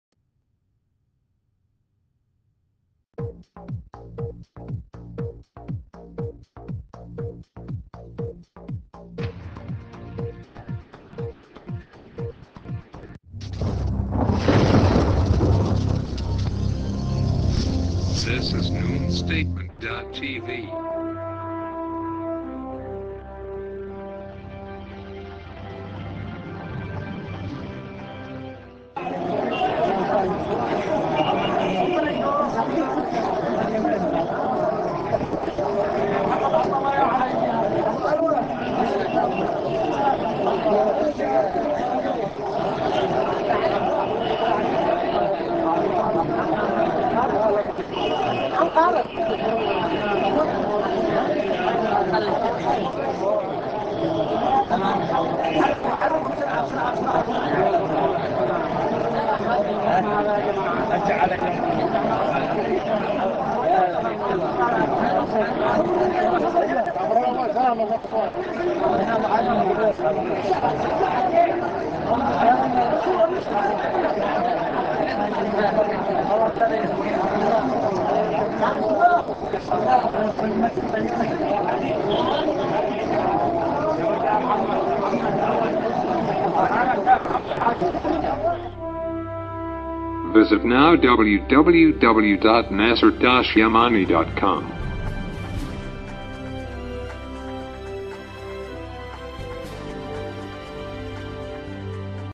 The Awaited Mahdi Imam Nasser Mohammad Al Yamani appeared among large crowds of supporters in Sanaa
The Awaited Mahdi Imam Nasser Mohammad Al-Yamani appeared among large crowds of supporters in Sanaa- Yemen